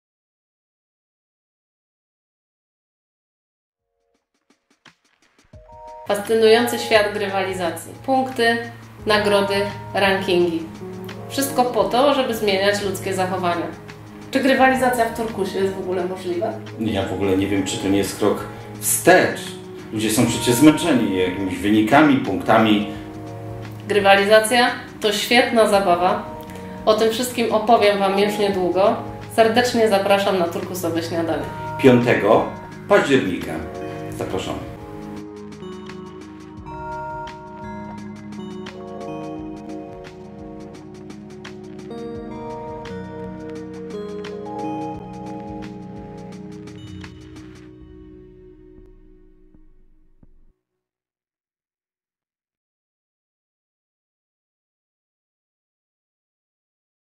Grywalizacja w Turkusie | Ada Dziopak-Strach
Turkusowe Śniadania Cieszyn mają ogromną przyjemność zaprosić was wszystkich na nasze nadchodzące bezpłatne zajęcia z grywalizacji. Czy w turkusie jest miejsce dla grywalizacji? W jaki sposób elementy rywalizacji mogą wpisać się w turkusową organizację? 💎Czy świat turkusu i grywalizacji mogą współistnieć, a jeśli tak, to w jaki sposób?
Ada Dziopak-Strach, jest specjalistką odpowiedzialną za HR z doświadczeniem w wielokulturowych/międzynarodowych zespołach.
Zajęcia są pełne spostrzeżeń, tipsów i wskazówek pogłębionych naukowo, bowiem Pani Ada wkrótce zakończy pisanie dysertacji doktorskiej właśnie na temat grywalizacji. Zajęć tych nie wypada przegapić.
Aby być częścią tego ciekawego wydarzenia, wystarczy być na bieżąco ze stroną
Uwaga: Udział w Śniadaniu jest całkowicie bezpłatny.